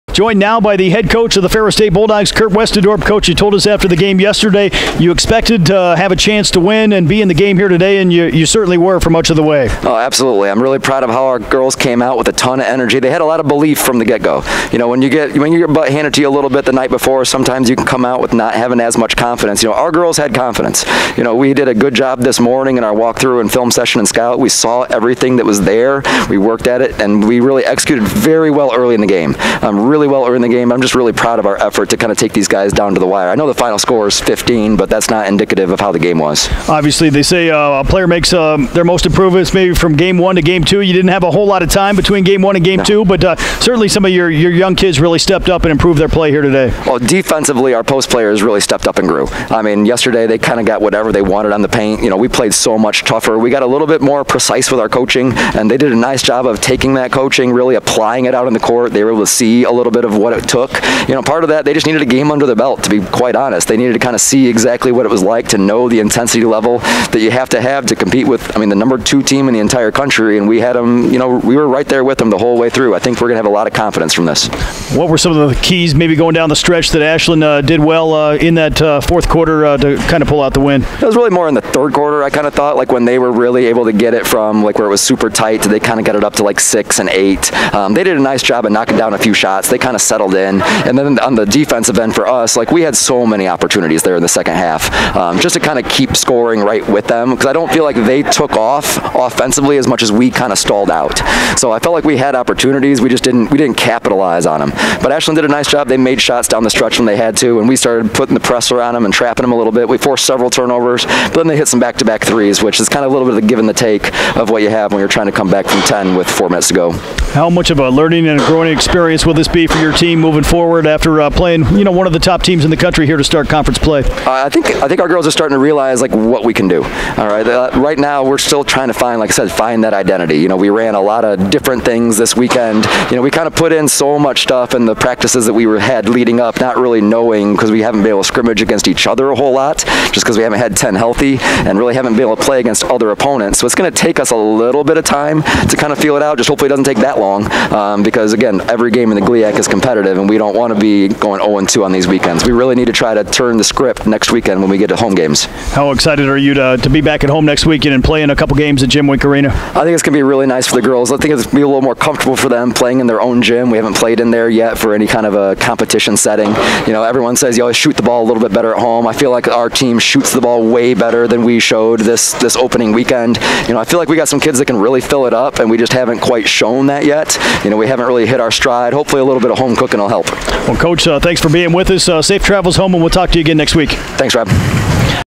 Women's Basketball Postgame vs Ashland
Postgame comments from FSU head coach Kurt Westendorp after the Bulldogs' 60-45 setback at #2 Ashland on Saturday.